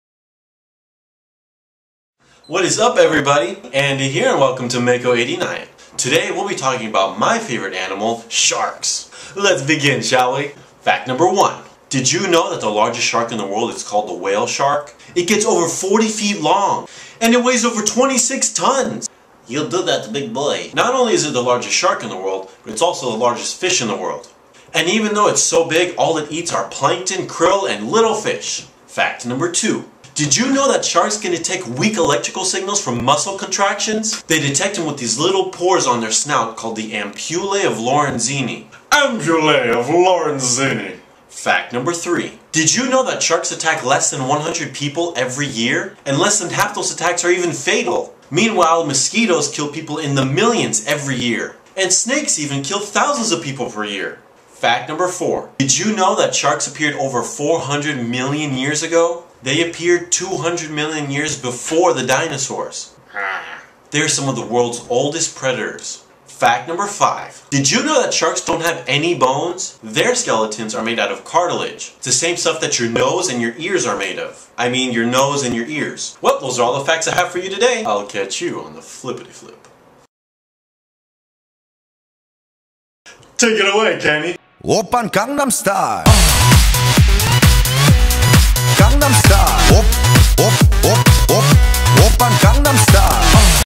Mako89: Sharks!!
Here are 5 facts about those ocean hunters, Sharks!!